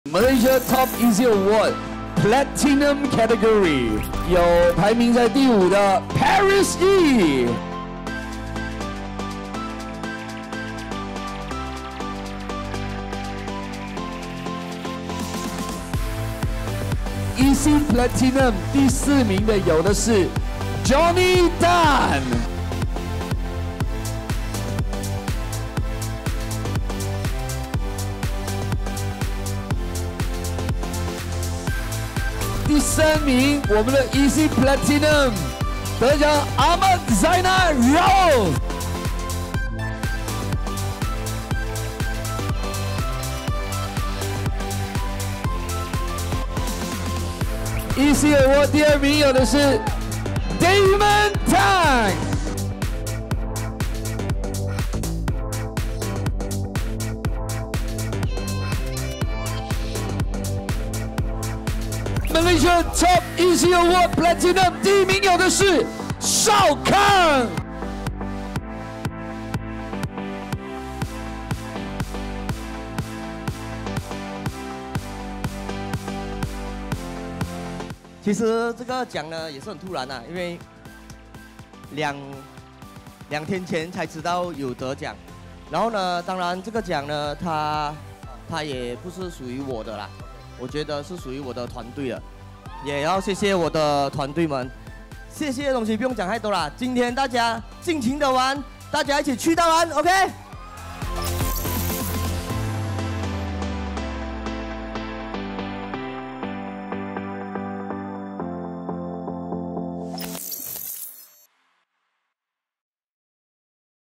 2022 SVO 3rd QUARTER QDM AWARD WINNERS - TOP EC PLATINUM
2022 SVO 3rd Quarter QDM Award Winners
⭐Top EC Platinum⭐

销售是企业的灵魂之一，✨SVO精英俱乐部白金系✨成员们朝着长远的目标，总是保持着激情🔥的状态带领团队伙伴们不断刷新业绩，一次次创造了销售的光辉成绩⚡。现在，就让我们以最热烈的掌声来恭贺Top 5的EC Platinum伙伴们。👏🏻

⭐TOP EC PLATINUM⭐
Top 1️⃣ Shao Kang
Top 2️⃣ Damon Tang
Top 3️⃣ Ahmad Zainal Rauf
Top 4️⃣ Johnny Tan
Top 5️⃣ Paris Yee

SVO - MAKE YOU POSSIBLE 🔥🔥🔥